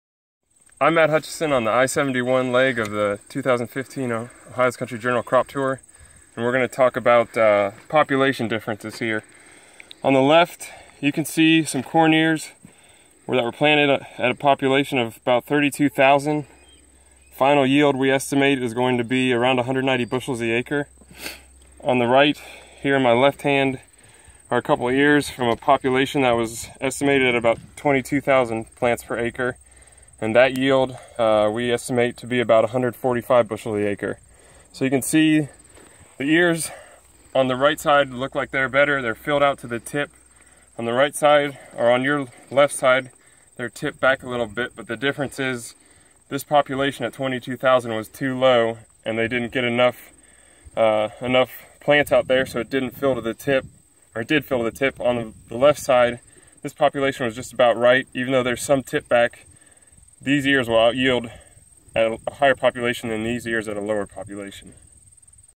Corn population vs yield
Matt Hutcheson is on the I71 leg of the 2015 Ohio Crop Tour and noticed a big difference in 2 areas of a Richland County corn field that will lead to a difference in yield.